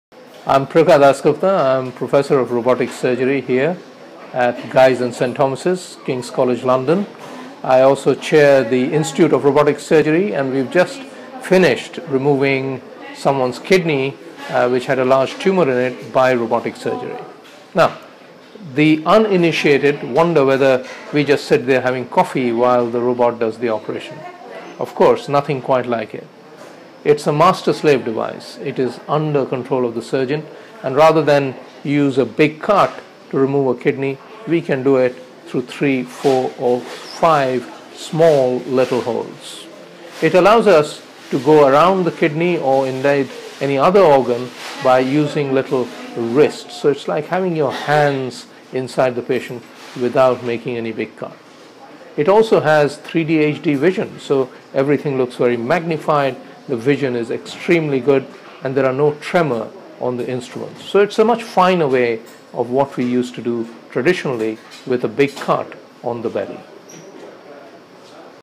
Prokar Dasgupta on robotic surgery
Professor Prokar Dasgupta explains the advantages of using robotic surgery.